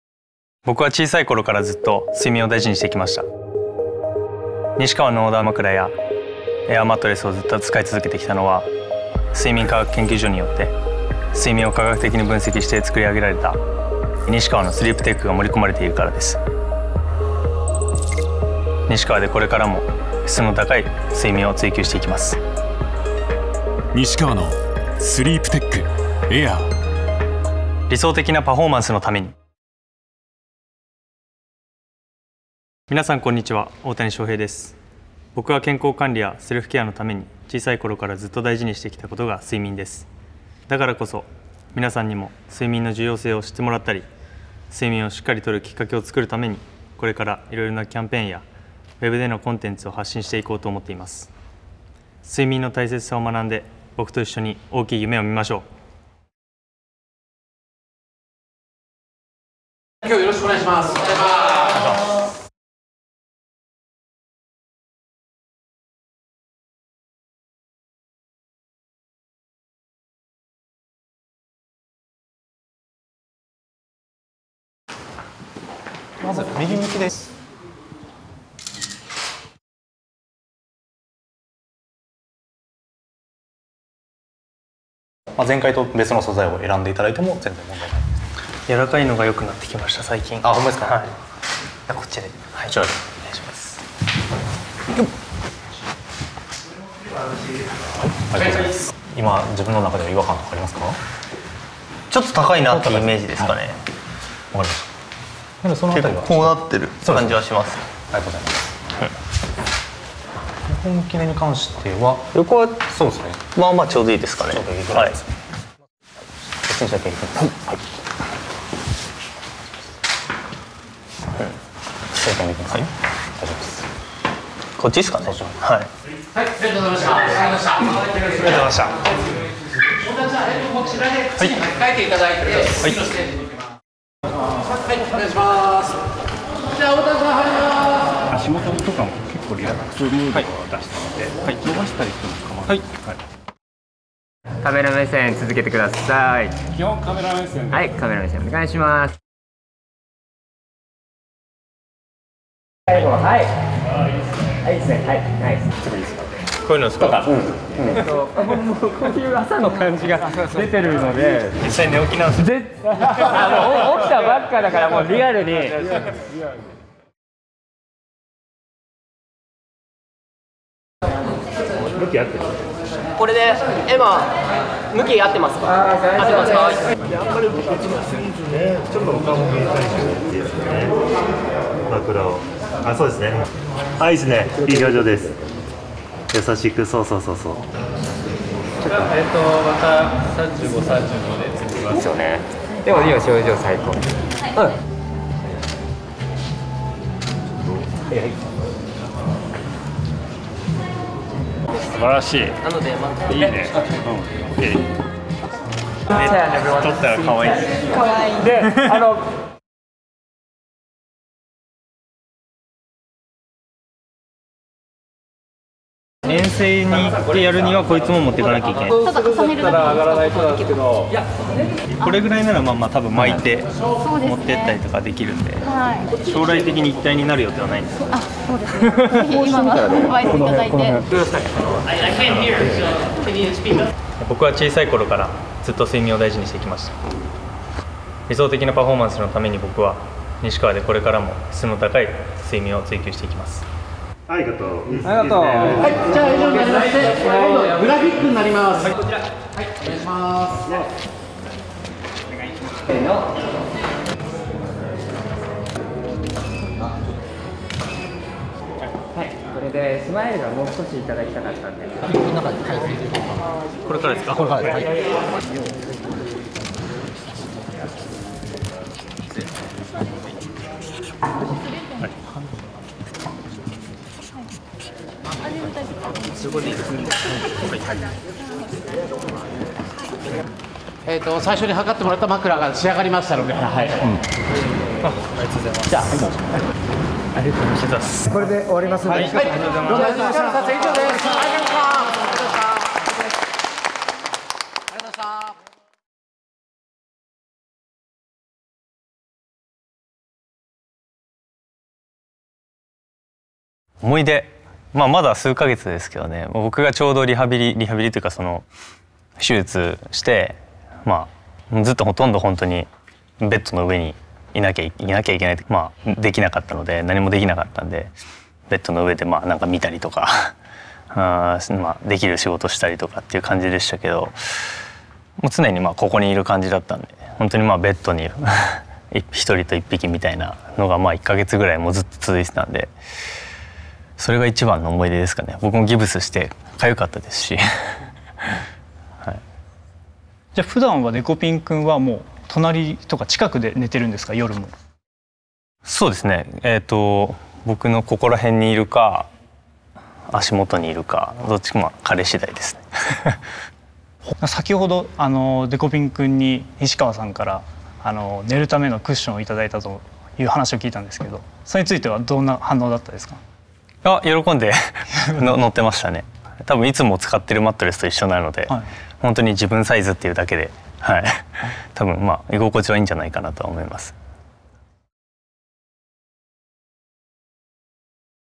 大谷翔平 出演『nishikawa エアー』新TVCM「エアー SLEEP TECH」篇＆メイキング・インタビュー
00:00 CM「エアー SLEEP TECH」篇 30秒Ver.
00:30 コメント
01:00 メイキング
04:59 インタビュー

【CM概要】
■タイトル：「エアー SLEEP TECH」篇（15秒・30秒）
■出演者：大谷翔平
■放送開始日：2024年3月13日（水）
■放送地域：全国